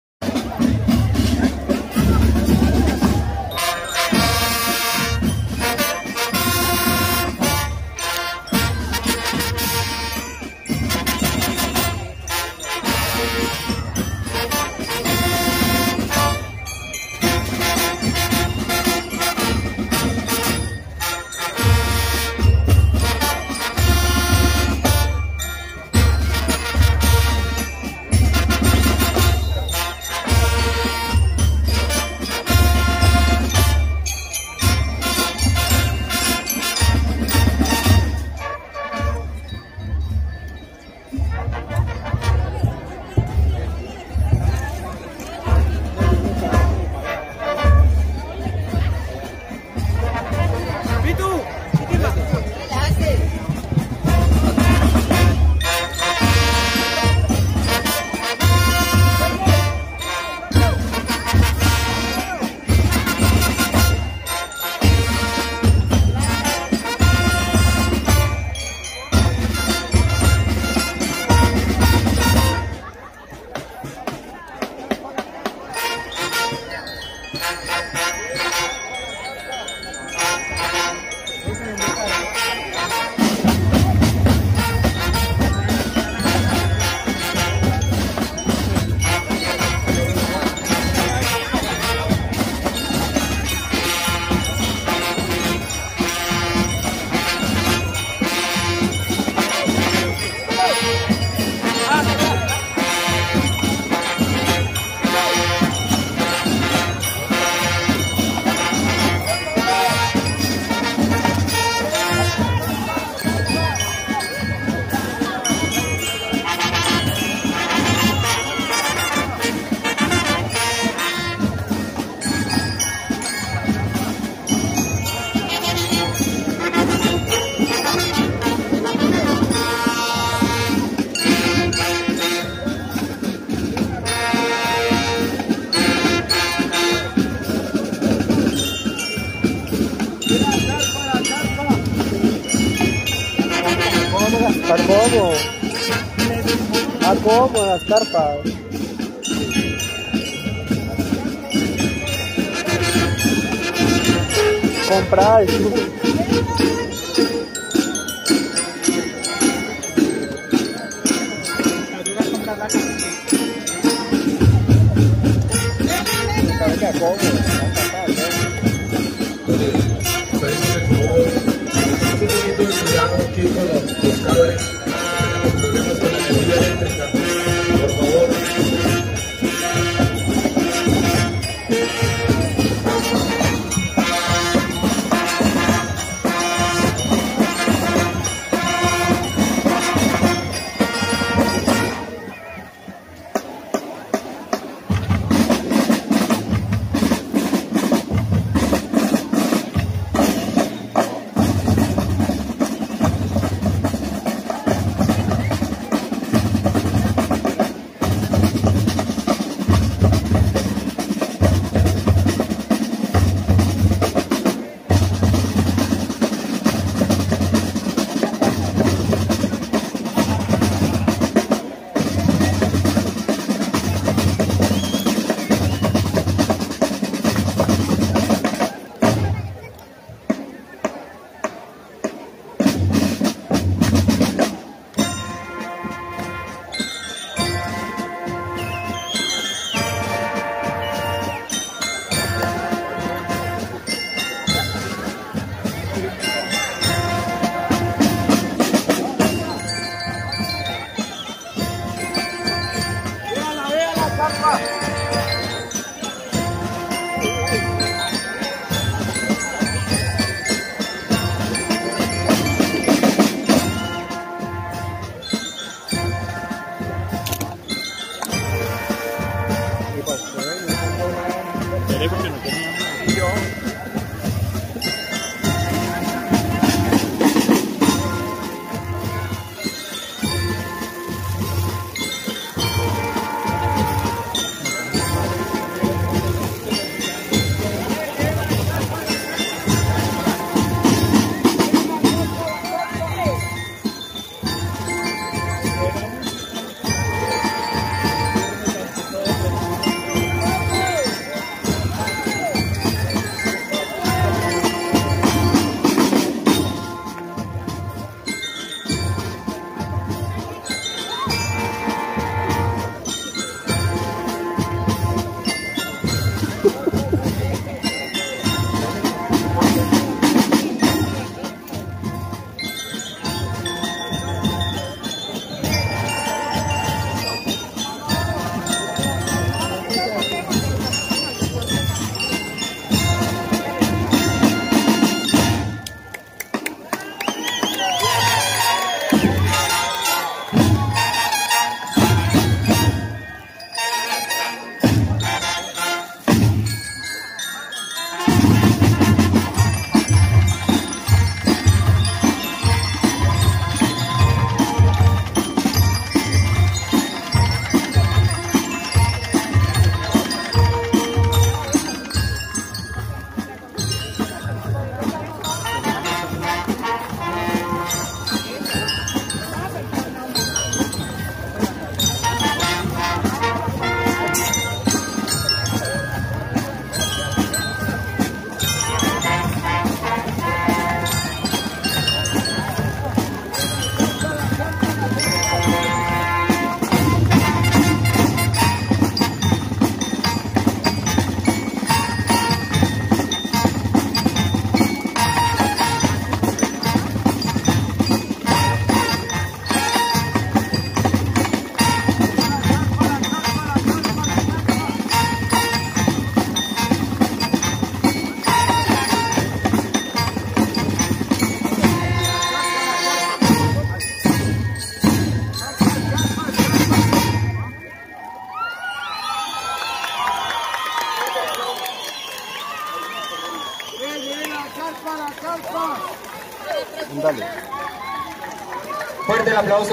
Banda Institucion Educativa Gustavo Matamoros D'Acosta - Calarca 2023
XII Festival y Concurso Nacional de Bandas Músico Marciales Paisaje Cultural Cafetero